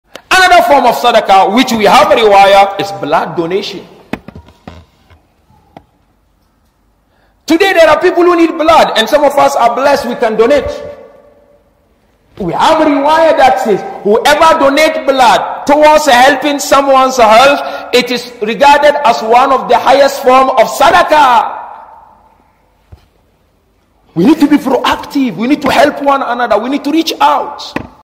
Sheikh Nur talks about Blood Donation during a Friday Khutba
On 12th February 2016, Sheikh Nur Muhammed delivered a Khutba in Nairobi discussing various aspects of working together to develop our community. During his talk, he mentioned blood donation as an important part of Sadaqah (charity).